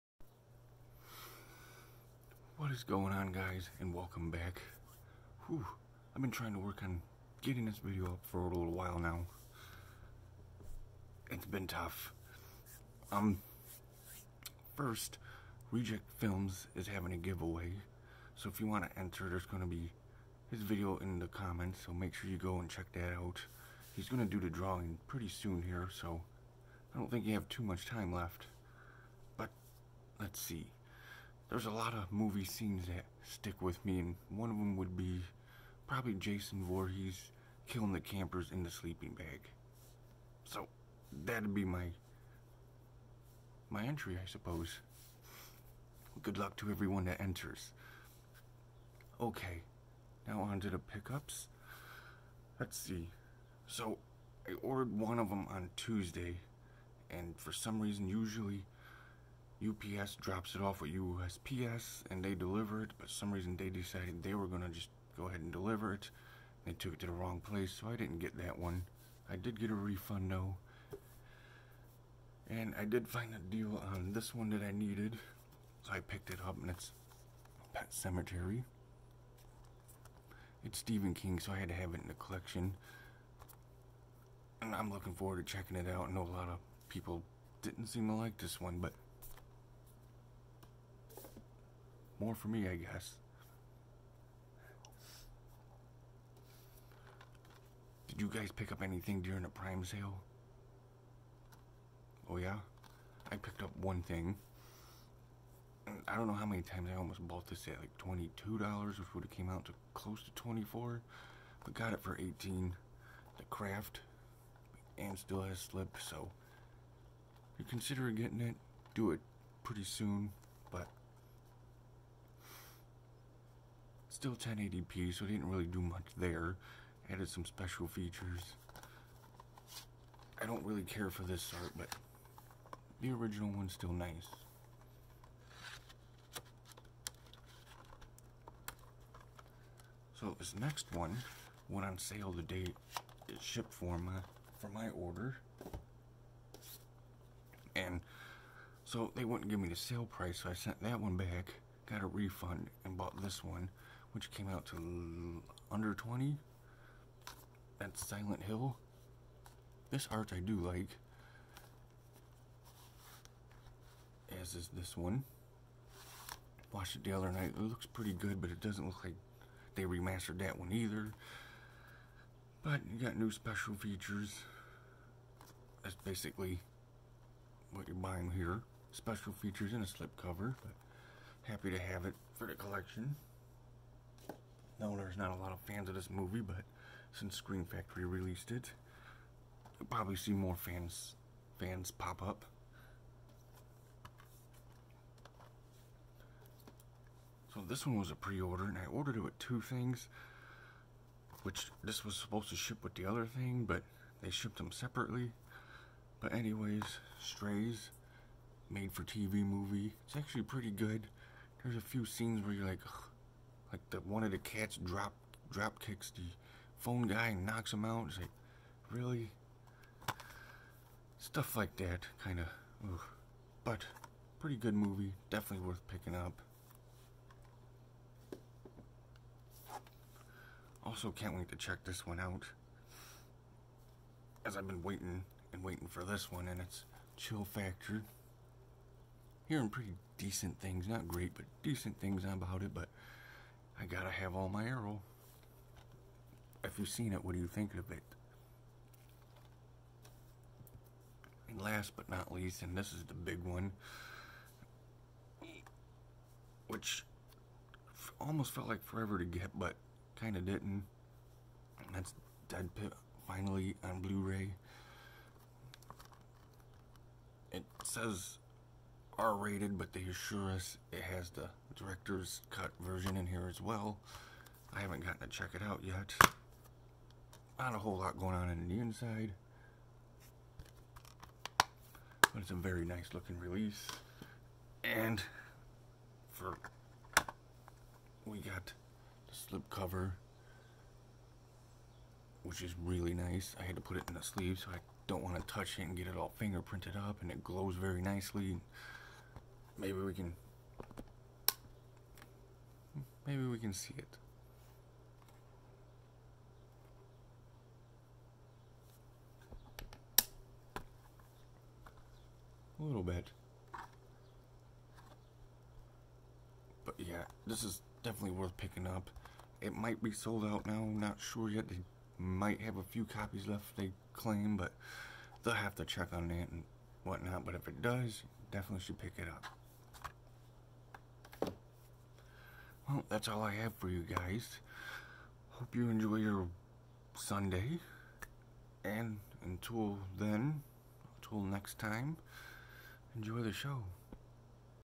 Episode 69: reject filmz contest entry & scream & arrow update.
Stop by reject filmz and give Dan a like an sub and be sure to enter his contest while you can.